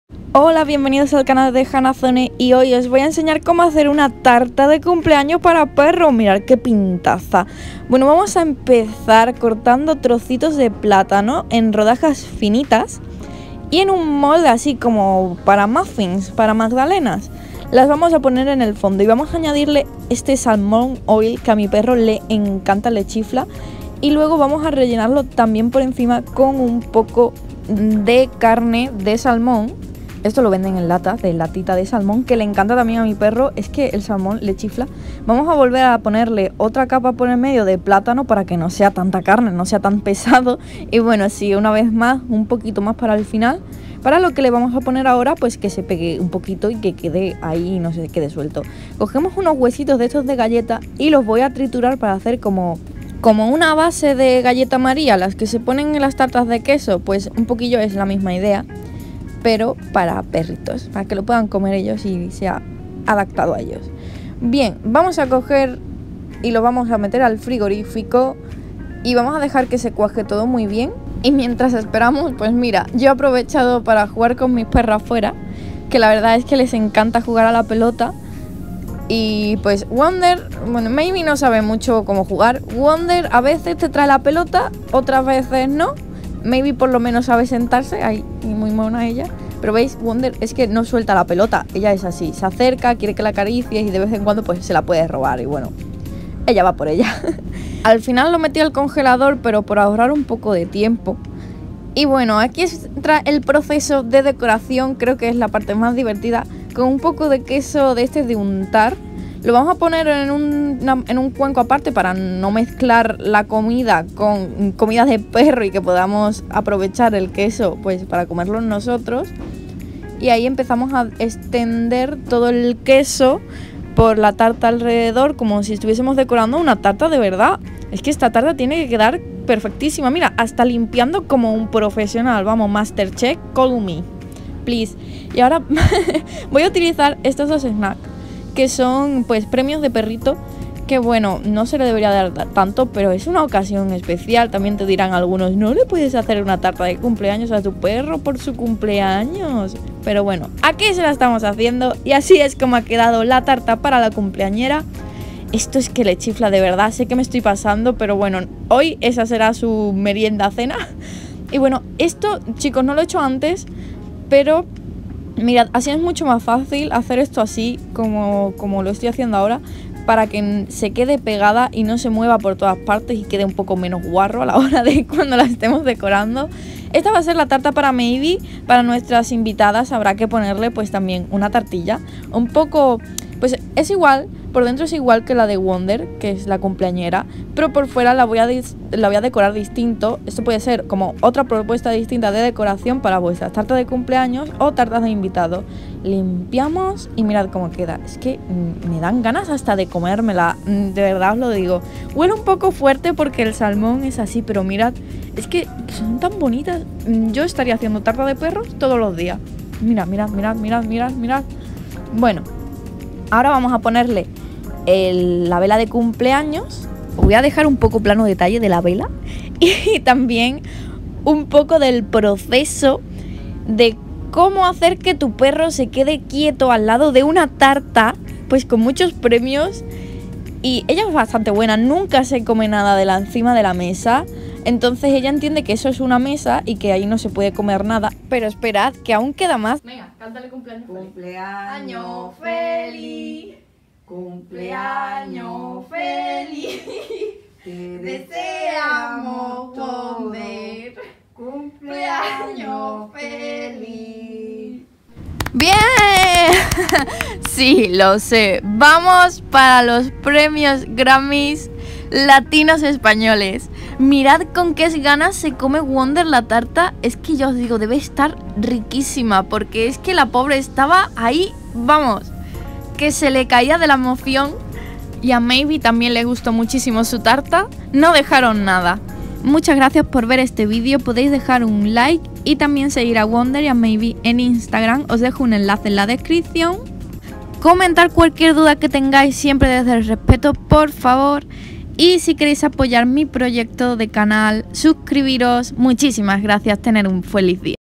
Cómo hacer una tarta para perro ✨Tarta de cumpleaños para perros✨
Wonder es un Cocker Spaniel Inglés y Maybe es una perro salchicha miniatura (O kaninchen). Para el 2 cumpleaños de mi perra Wonder, he hecho estas deliciosas tartasparaperros con las que podrán disfrutar hasta los invitados.

Os dejo los ingredientes:
Chuches variadas
Salmon oil
Lata de salmón

Set para cumpleaños

Y si os da pereza cocinar, ya las venden hechas:
Tarta para perro
Tarta rosa

¡Siempre puedes animarte a cocinarles algo! Cualquier cosa que decidas, seguro les gusta :)